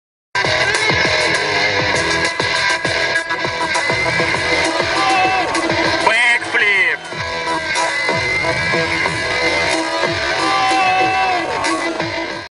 Ragazza scambia la rampa per sci con passerella di moda
» Seguiteci su - gli ultimi video virali e divertenti da tutto il mondo